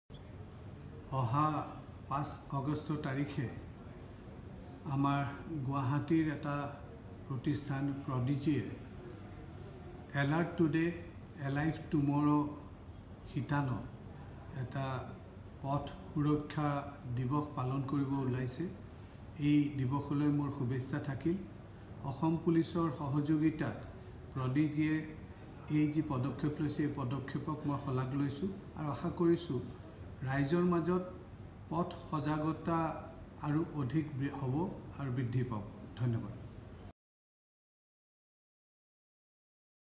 SHRI SIDDARTH BHATTACHARYA
Shri Siddarth Bhattacharya, Cabinet Minister of Education, Guwahati Development, Law & Justice Department, Government of Assam speak in support of Prodigy's campaign on Traffic and Road Safety Awareness- ALERT TODAY, ALIVE TOMORROW.